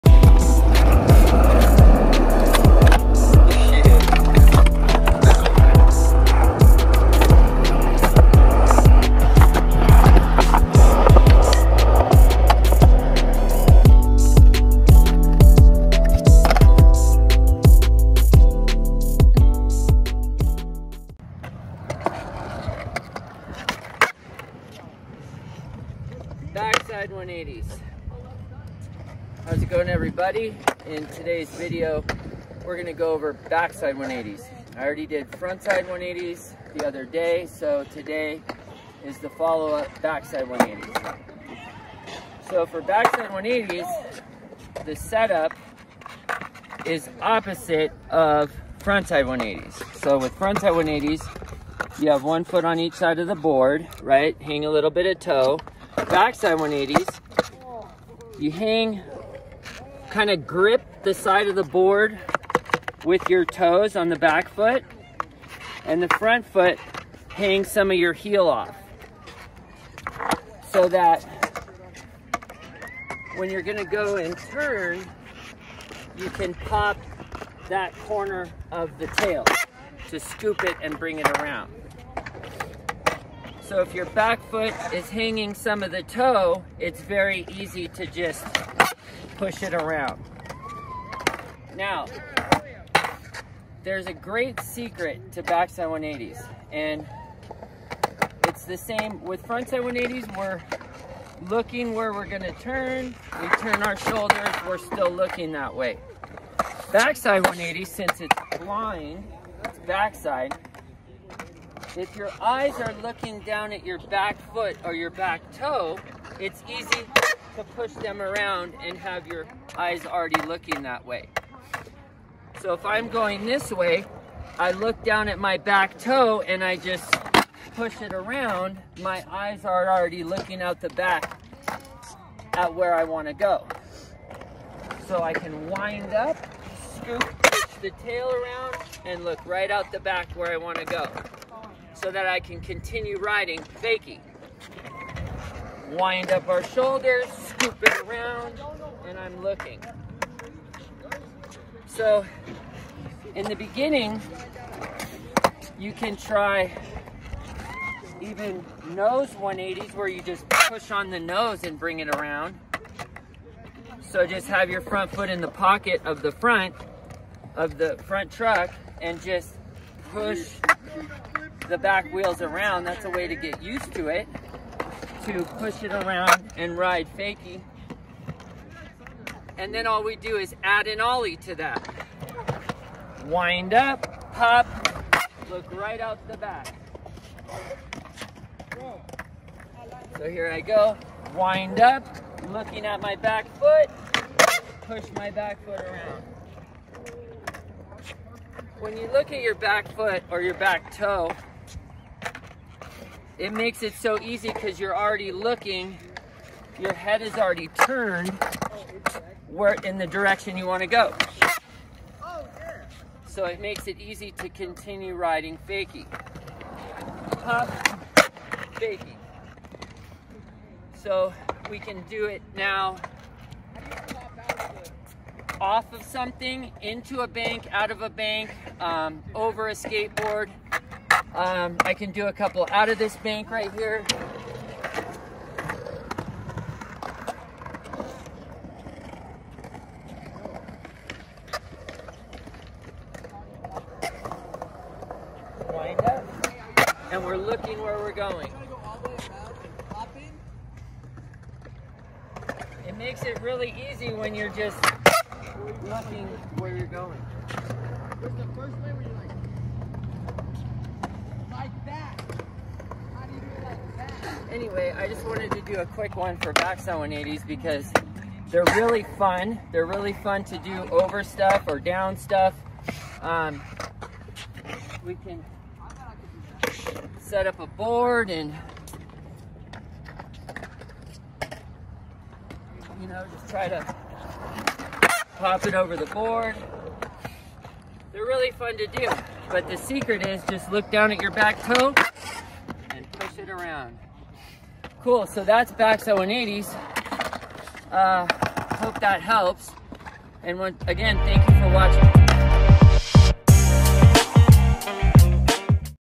The SECRET to BACKSIDE 180s
In this video you will be learning how to do backside 180s on you skateboard